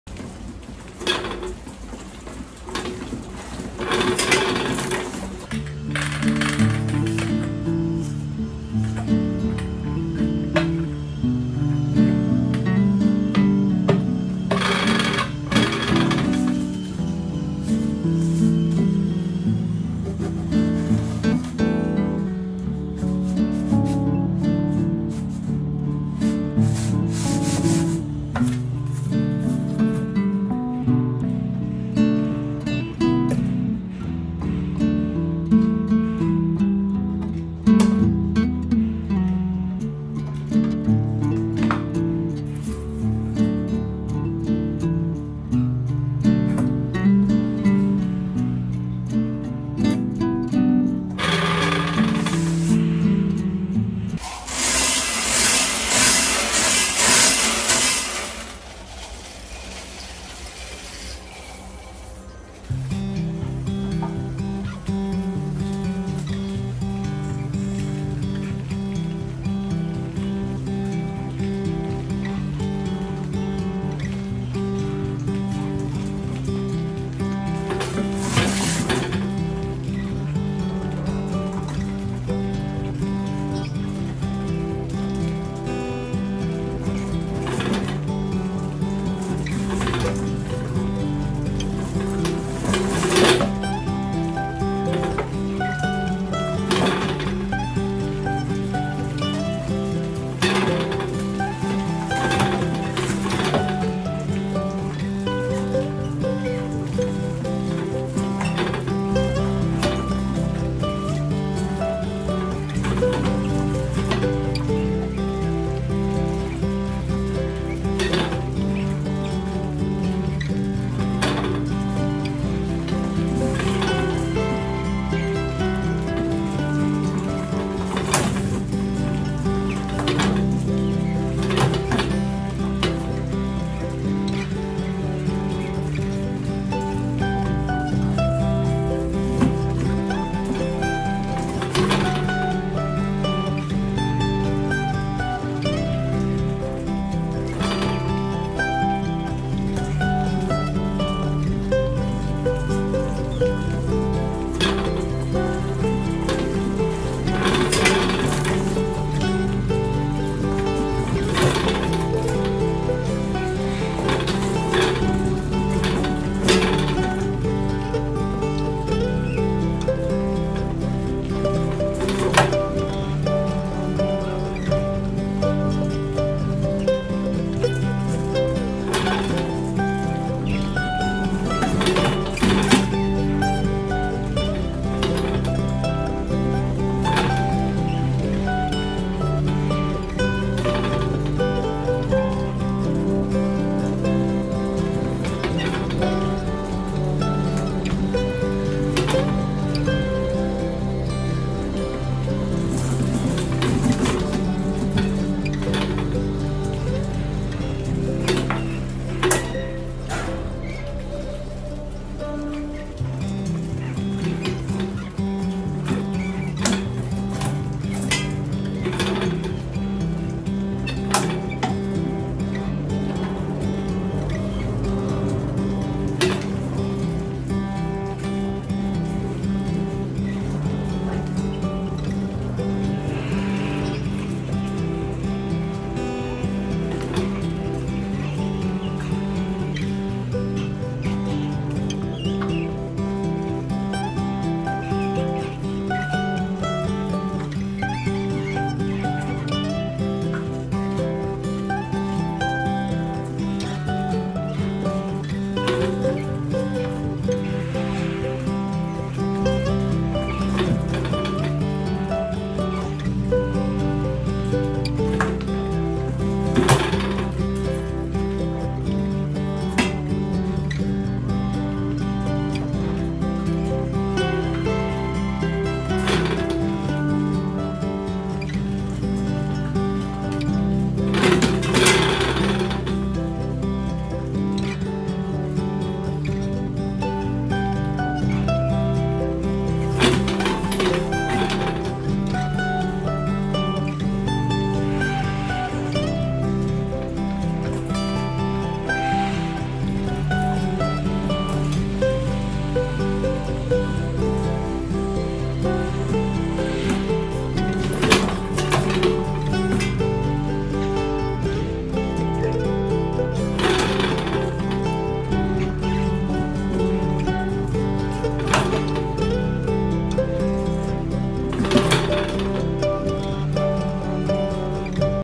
Mouse trap \ Electric mouse trap \ Extremely effective mouse trap with beer cans
In this video, we test one of the most popular mouse traps on the market today.
We want to bring you useful videos.